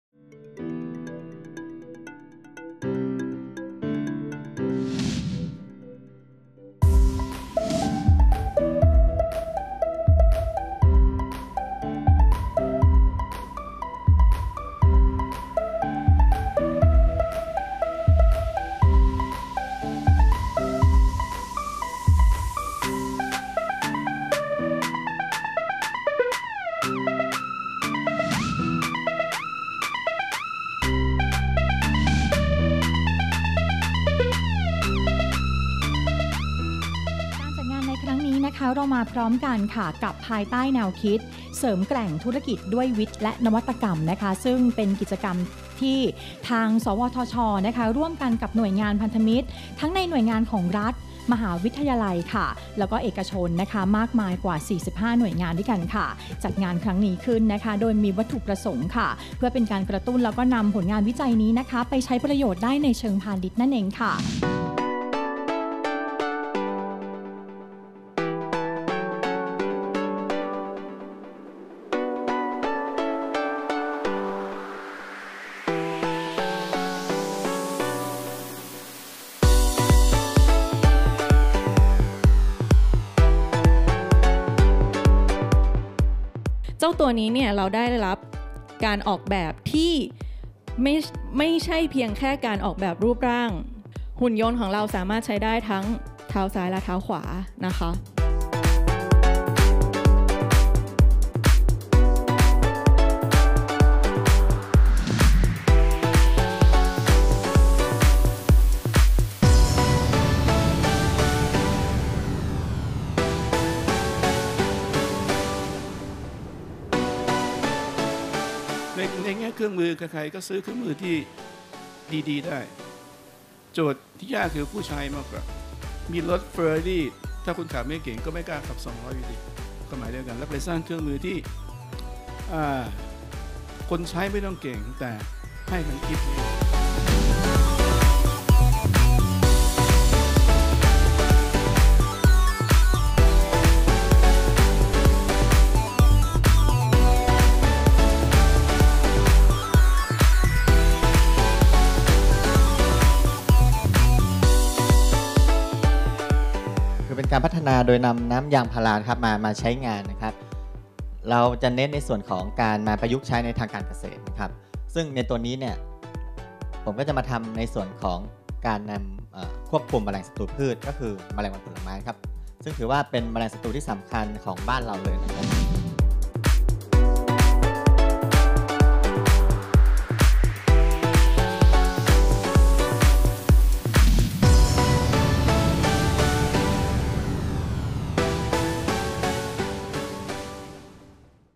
ประมวลภาพการนำเสนอผลงาน ภายในงาน Thailand Tech Show 2018
ประมวลภาพการนำเสนอผลงาน ภายในงาน Thailand Tech Show 2018 ที่จัดขึ้นระหว่างวันที่ 4 - 8 กรกฎาคม 2561 ณ ศูนย์นิทรรศการและการประชุมไบเทค บางนา กรุงเทพฯ